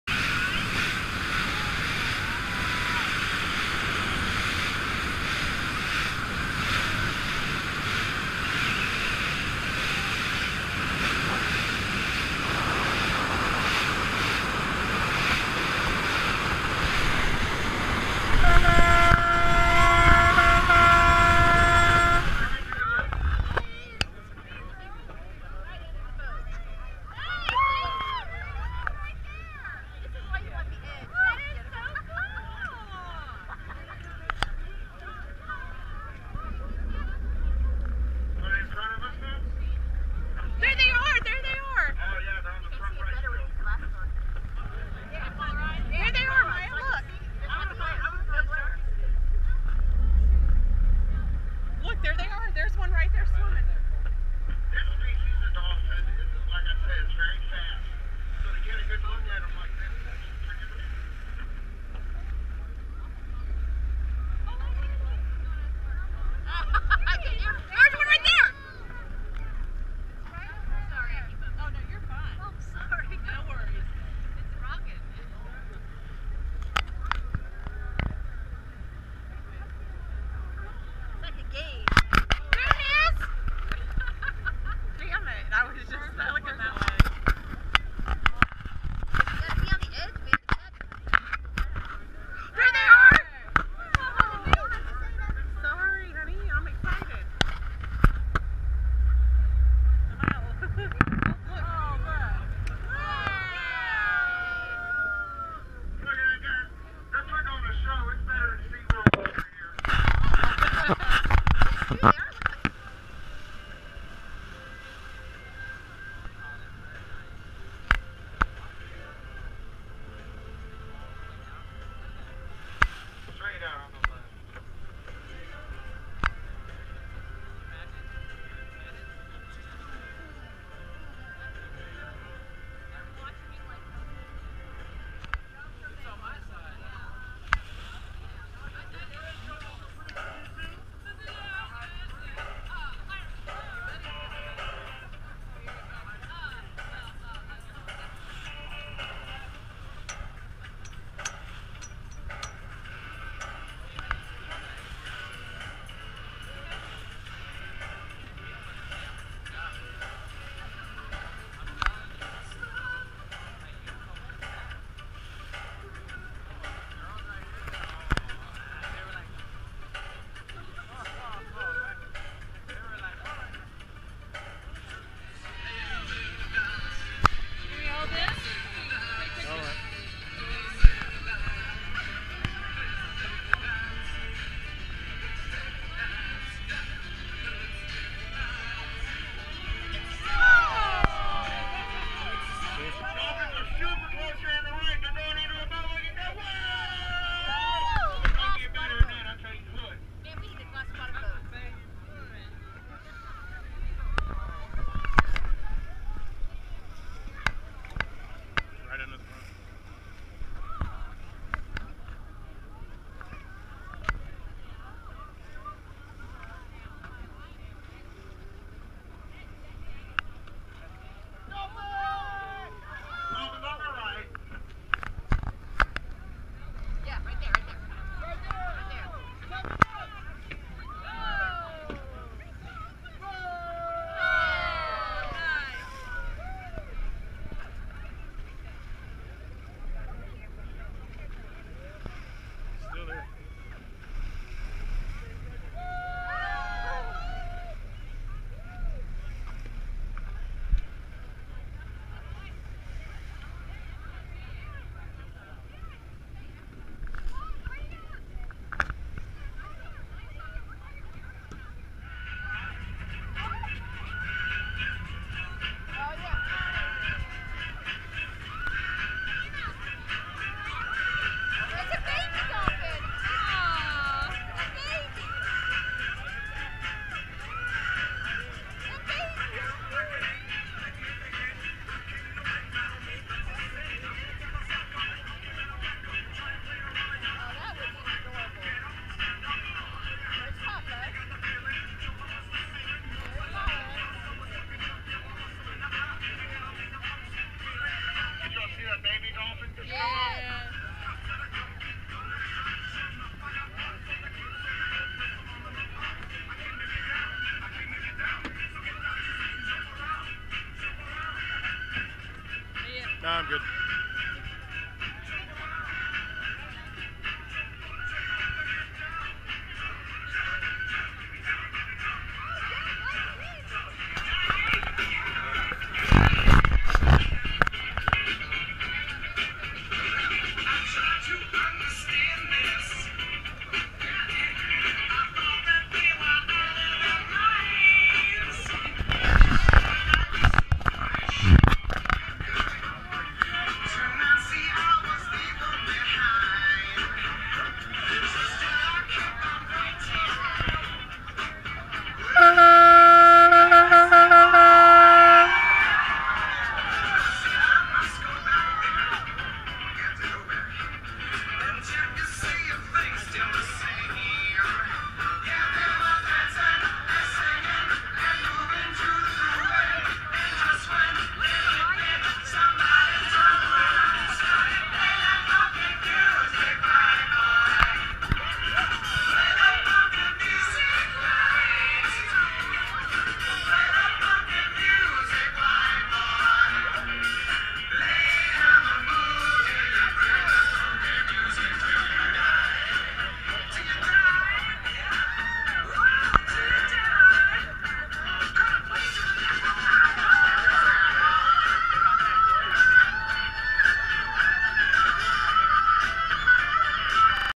Dolphin Cruise 2016
Dolphin Cruise on the Sea Blaster in Destin Florida.